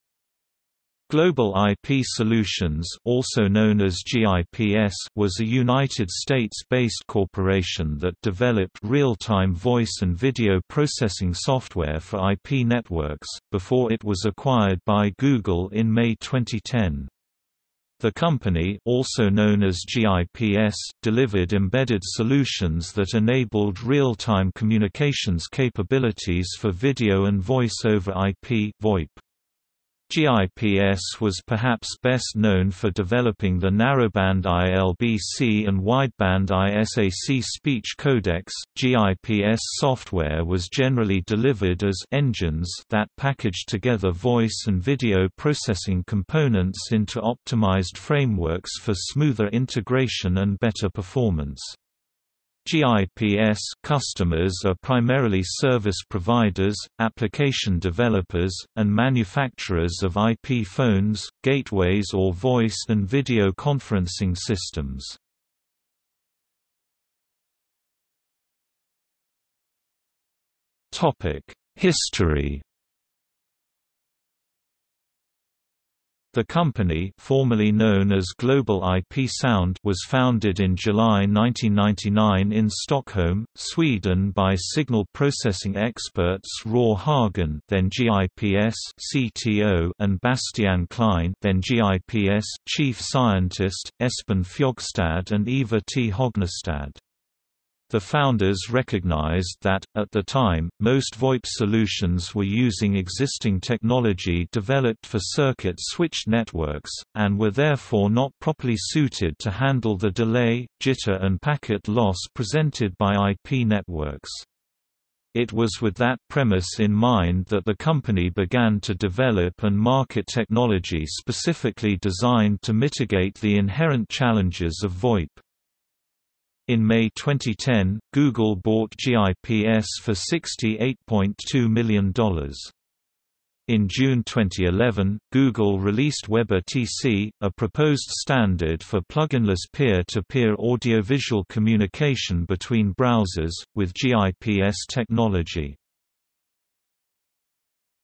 Global IP Solutions | Wikipedia audio article
00:01:07 History

- Socrates

SUMMARY
Global IP Solutions (also known as GIPS) was a United States-based corporation that developed real-time voice and video processing software for IP networks, before it was acquired by Google in May 2010. The company (also known as GIPS) delivered embedded solutions that enabled real-time communications capabilities for video and Voice over IP (VoIP). GIPS was perhaps best known for developing the narrowband iLBC and wideband iSAC speech codecs.GIPS software was generally delivered as “engines” that packaged together voice and video processing components into optimized frameworks for smoother integration and better performance. GIPS’ customers are primarily service providers, application developers, and manufacturers of IP phones, gateways or voice and video conferencing systems.